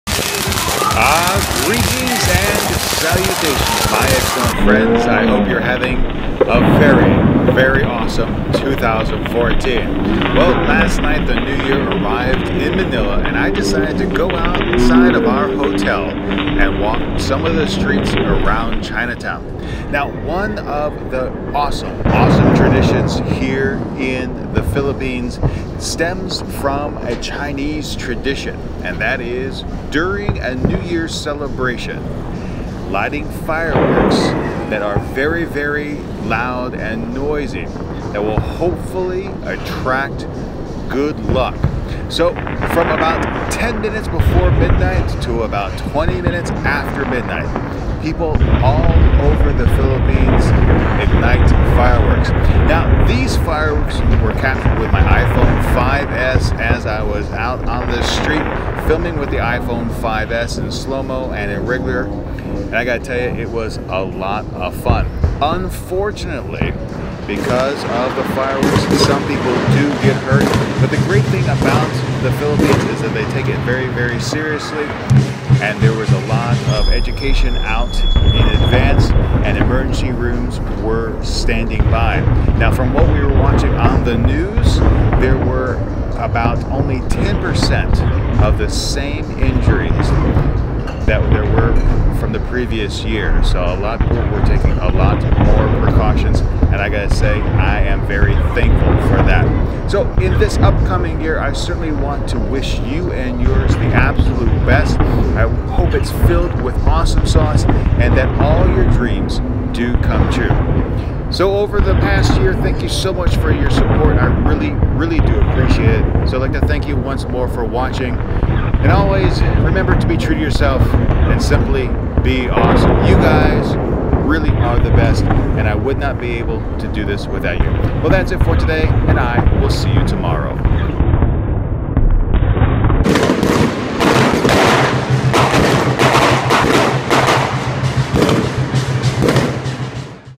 Happy New Year 2014
Thank you for all your support in 2013. I wish you the best in 2014. May your year be filled with awesomesauce.

I filmed the following video with the iPhone 5S to take a look at the tradition of lighting fireworks for good luck.